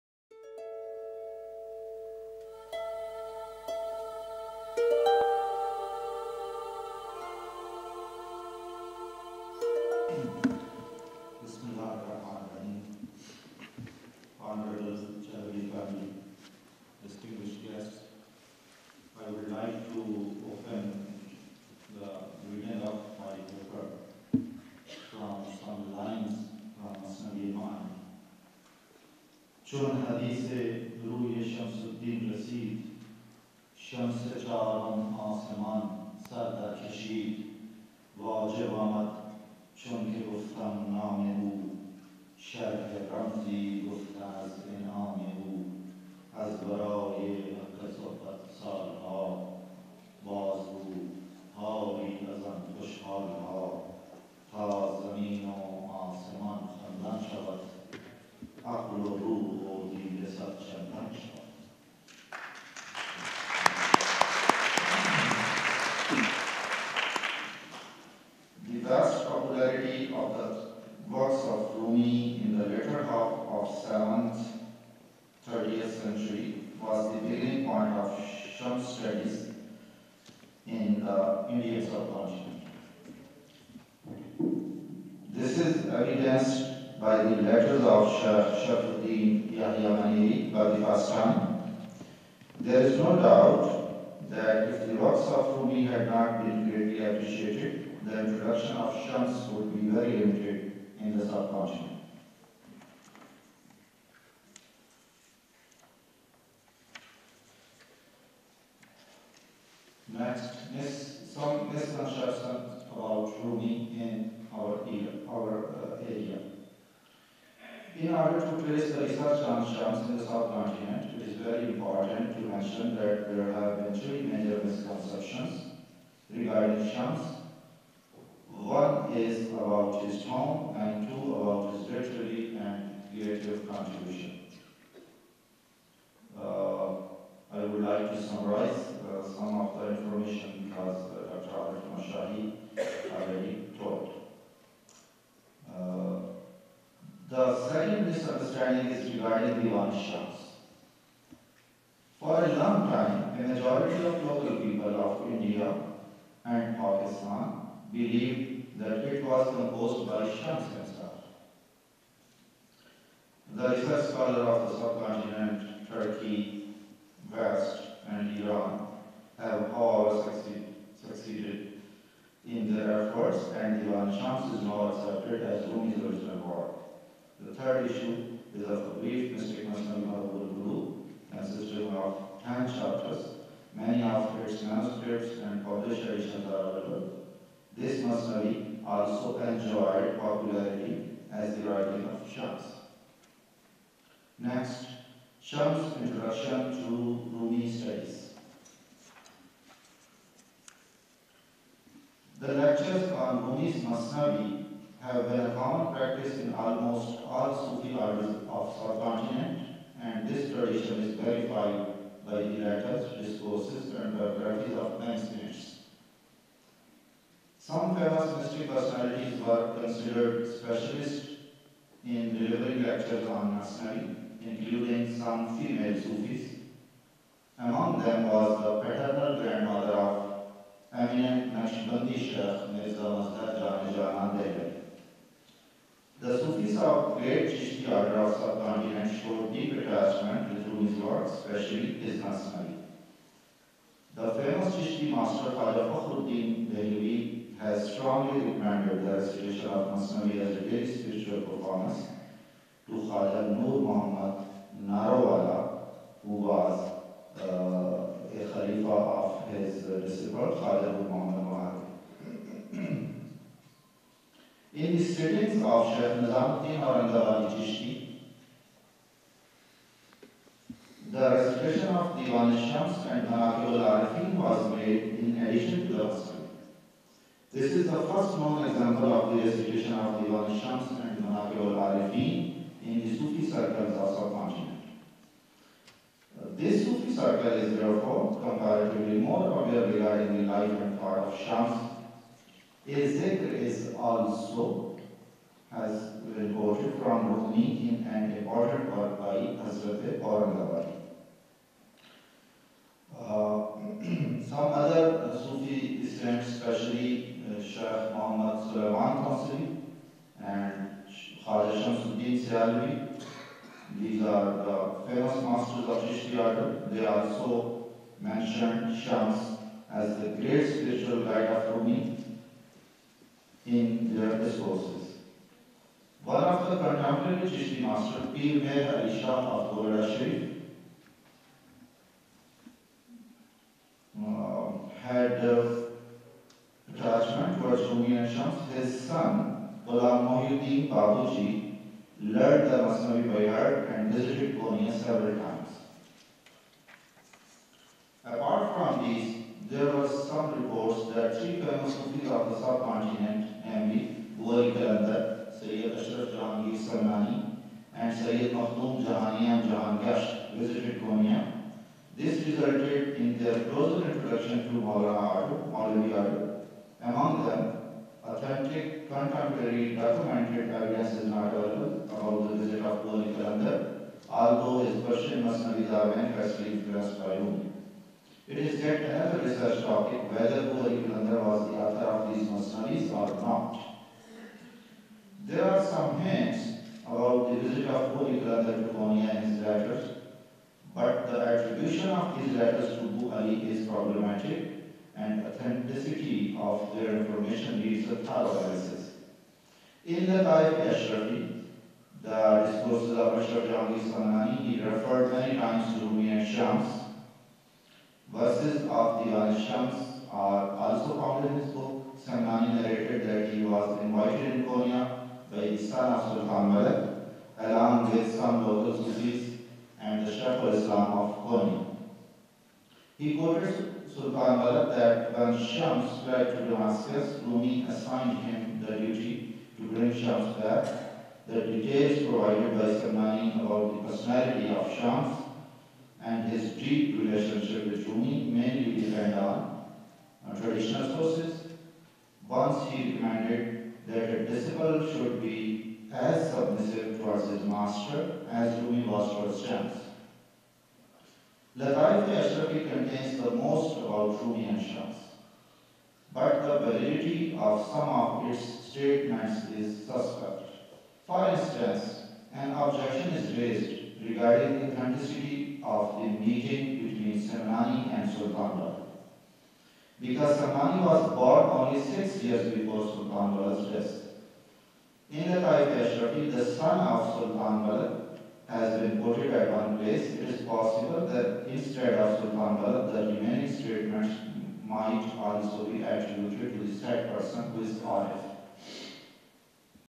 Hz. Şems Semp. Konya Ghulam Moeen Uddin-1
Hz. Şems Sempozyumu Konya Tebliğleri-  Dr. Ghulam Moeen Uddin "Alt Kıtada Şems-i Tebrizi Üstüne Araştırmalar" "Studies on Shams-e Tabrizi in the Sub- Continent"-1